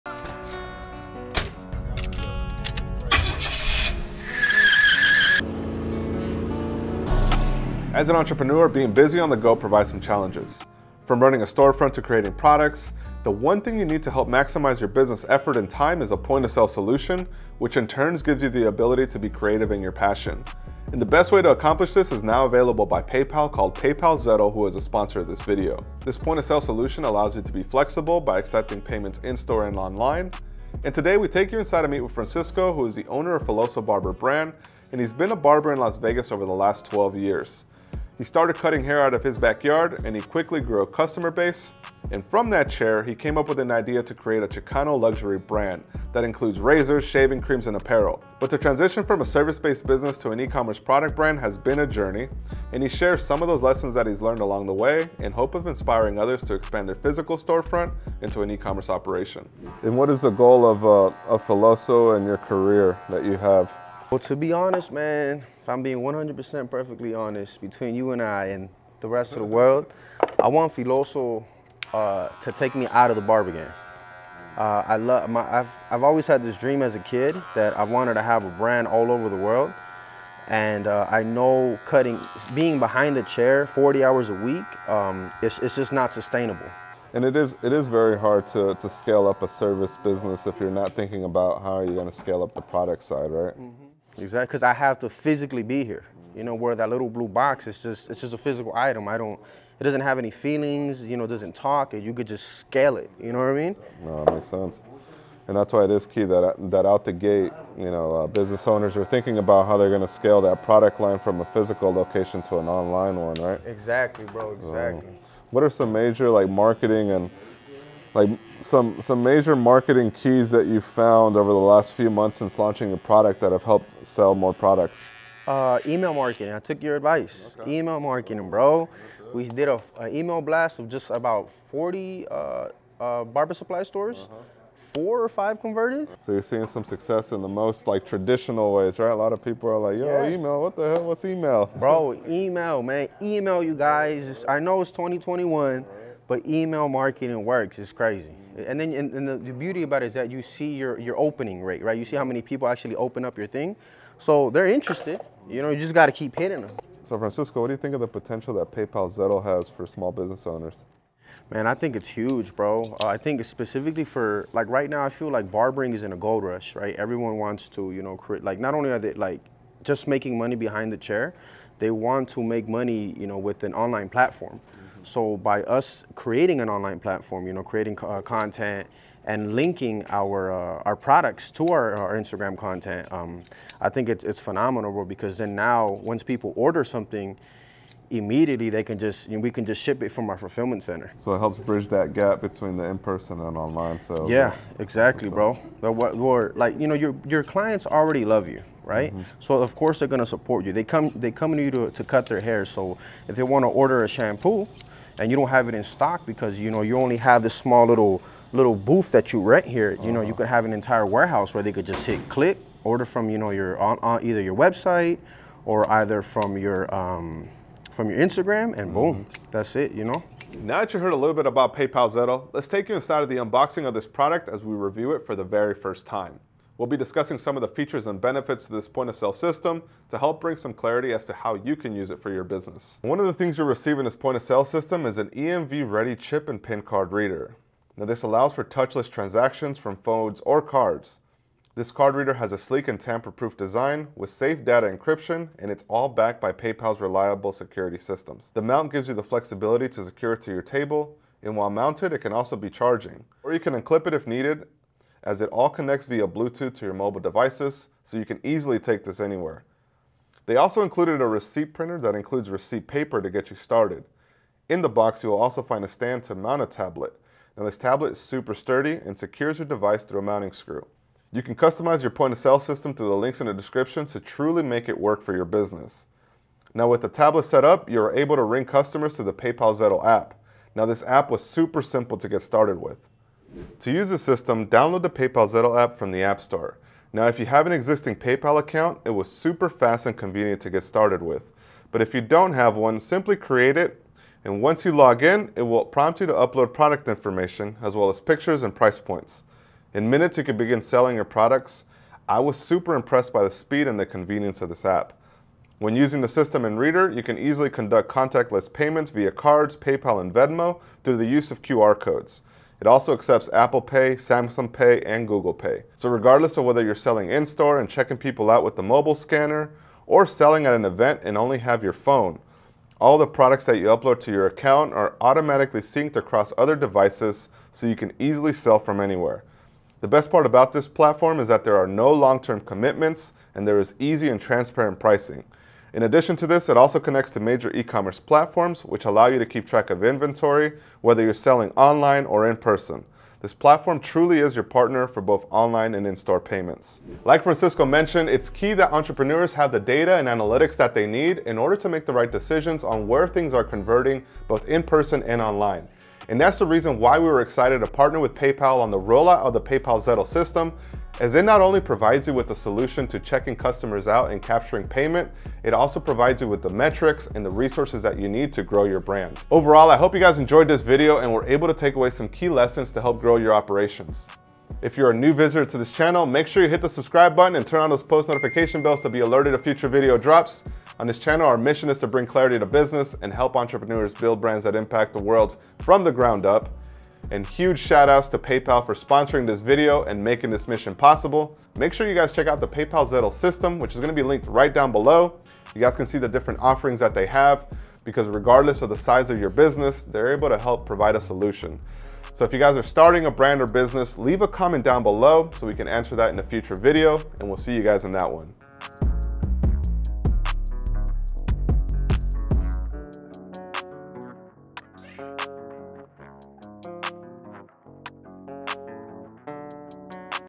The Best POS System to EASILY Sell Products In Person And Online
PayPalPartner Are you an entrepreneur looking to scale your business? In this video, we show you a POS system that helps you simplify and streamline your workday by managing the business you do both online and in-person for the entrepreneur on the go!

If you’re an entrepreneur or small business owner, it’s essential that you have a point-of-sale system to accept payments and track and manage the sales of your company. Without these systems in place, you’re bound to waste time, money, and resources. If you’re just starting up it can feel difficult to find a system that works for you, especially without the budget of a larger company. PayPal Zettle gives small business owners the luxury of taking all types of payments in-person and tracking and managing their sales – all in one account. What this means for you and me is that we can sell in person and online without having to think twice about inventory or how we’re getting paid.

Scale Your Business today!

Get your FREE Blueprint 📲 and 10 Step Checklist to quickly start your clothing line

📚Chapters:
0:00 The Best POS System For Your Business
1:01 Starting a Barber Brand From The Ground Up
1:56 The One Major Tips For Marketing Your Business
2:38 The Potential PayPal Zettle Has For Small Business Owners
3:41 Reviewing PayPal Zettle System
4:43 How The Paypal Zettle System Works